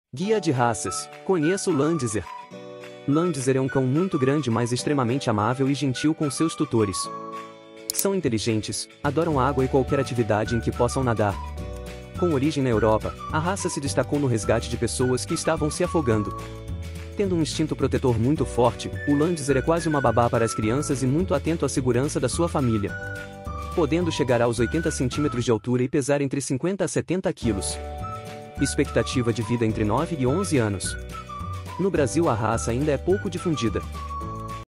Guia de raças - Conheça o Landseer! # shorts
Guia de raças - Conheça o Landseer!

Landseer é um cão muito grande mas extremamente amável e gentil com seus tutores.
São inteligentes, adoram água e qualquer atividade em que possam nadar. Com origem na Europa, a raça se destacou no resgate de pessoas que estavam se afogando.
Com um instinto protetor muito forte, o landseer é quase uma babá para as crianças e muito atento a segurança da sua família.
Podendo chegar aos 80 centímetros de altura e pesar entre 50 a 70 quilos.
Expectativa de vida entre 9 e 11 anos.
No Brasil a raça ainda é pouco difundida.
guia de raças,
landseer dog